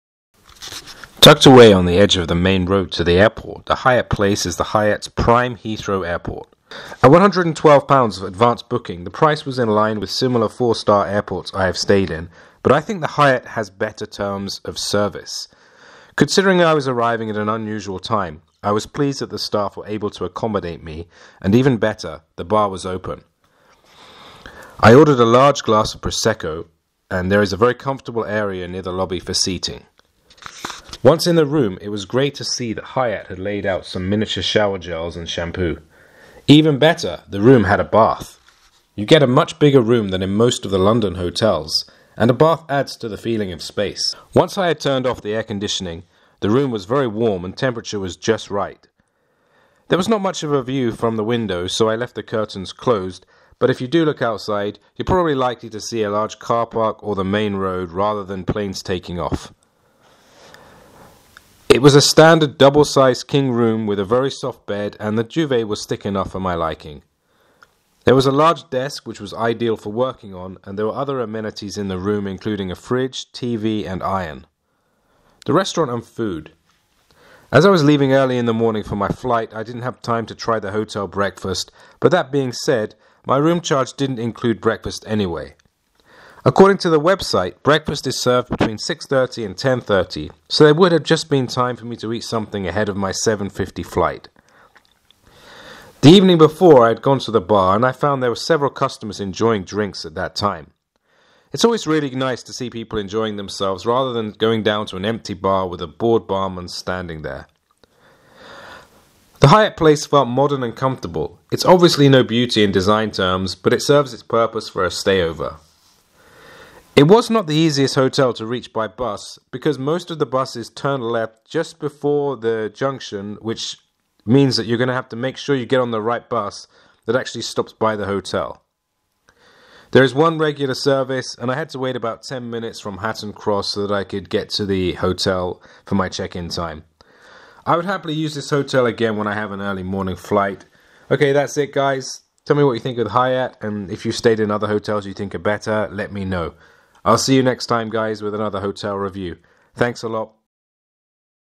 Hotel VLOG REVIEW Hyatt Place Heathrow Airport Bath Road
A regular video review where I look at a hotel in detail.